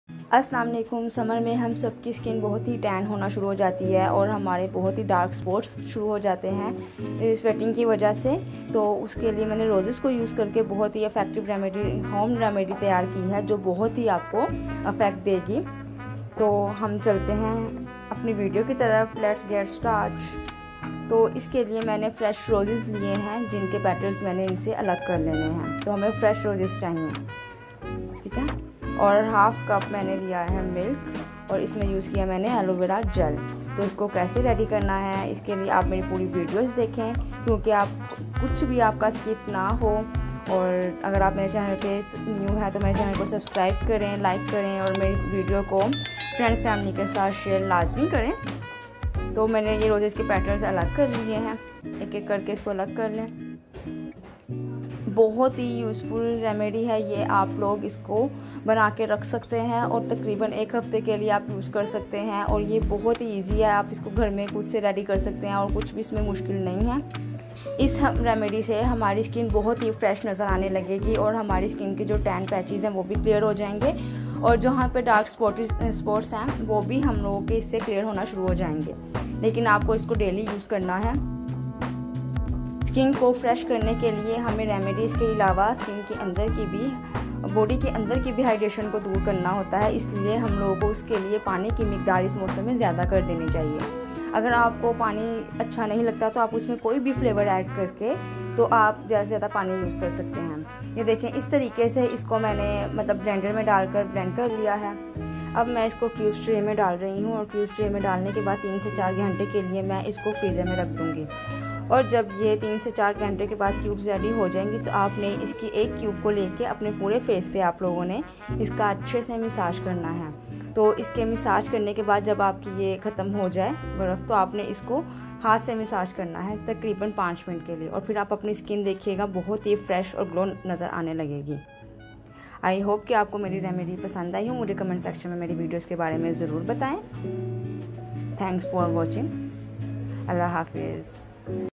DIY Rose petals face beauty tips || Rose face secret for summer || By Meki
Roses use,  Rose petals,  Rose petals for face,

Ingredients :
Rosepetals
milk
aloeveragel